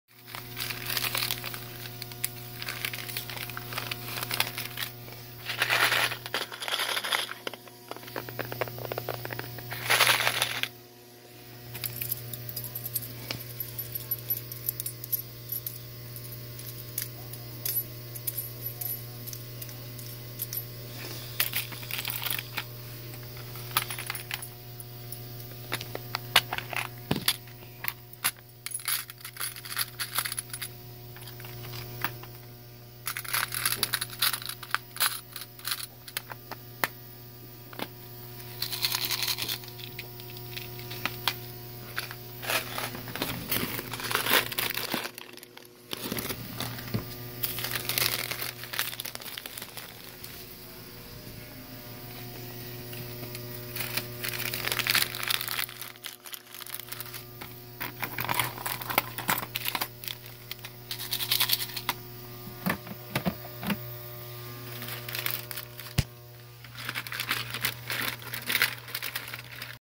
1 minute ASMR Organizing at work
I was able to slip more of this. usually my job is loud. so I try to keep this quiet for yall. enjoy :)